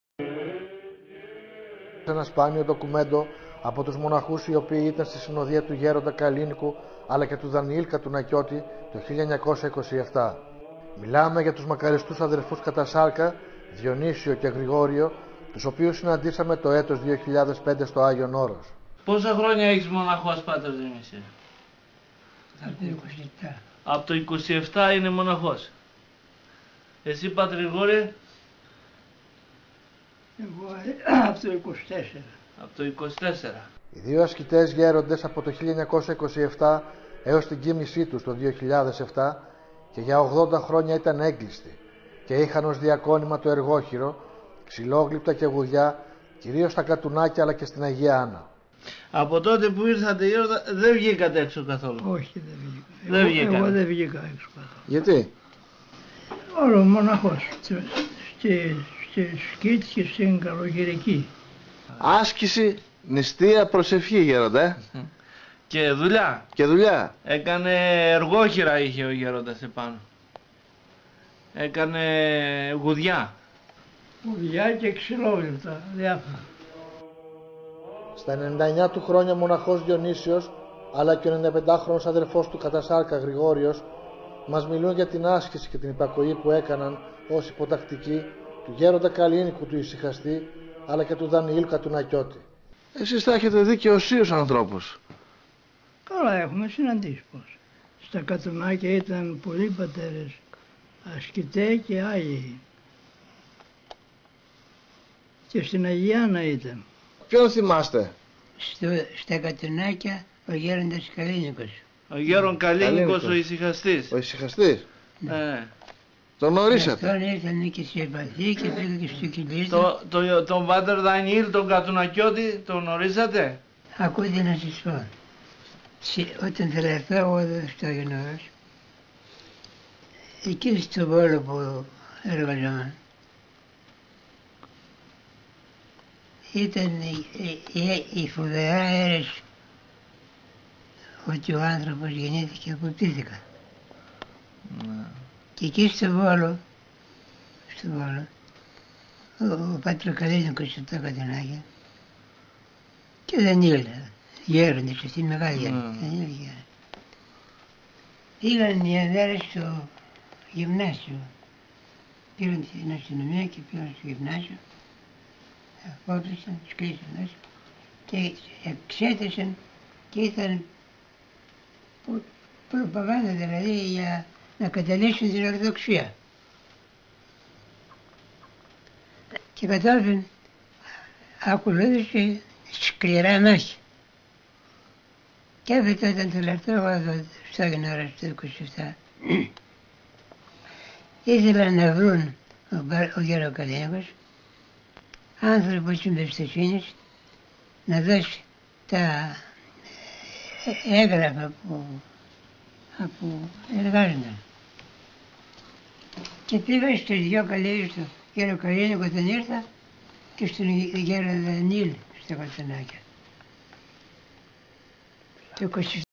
ΒΙΝΤΕΟ ΝΤΟΚΟΥΜΕΝΤΟ: Τα αδέρφια ασκητές μοναχοί στην συνοδεία του Δανιήλ Κατουνακιώτη -ΑΓΙΟΝ ΟΡΟΣ
ΑΓΙΟΝ ΟΡΟΣ -ΑΣΚΗΤΕΣ

Ενα  σπάνιο ντοκουμέντο φέρνει στο φώς ο δημοσιογράφος Δημήτρης Στρουμπάκος  από το Αγιον Ορος.

Τους δύο έγκλειστους  Μοναχούς -Ασκητές μακαριστούς  Διονύσιο και  Γρηγόριο οι οποίοι το 1927 πήγαν στην συνοδεία του Γέροντος Καλλίνικου του Ησυχαστή και του Γέροντα Δανιήλ του Κατουνακιώτη. Στα 99 του χρόνια ο Μοναχός Διονύσιος και στα 95 του χρόνια ο κατά σάρκα αδερφός του Γρηγόριος μιλούν για την εγκαταβίωσή τους στα Κατουνάκια, για την γνωριμία τους με τους Οσίους Γεροντάδες Καλλίνικο και Δανιήλ Κατουνακιώτη, για το εργόχειρό τους αλλά και για το πνευματικό νόημα του Μοναχισμού. Τό γύρισμα έγινε το 2005 στο Ιατρείο της Ιεράς Μονής Εσφιγμένου όπου και φιλοξενούνταν οι δύο Μοναχοί.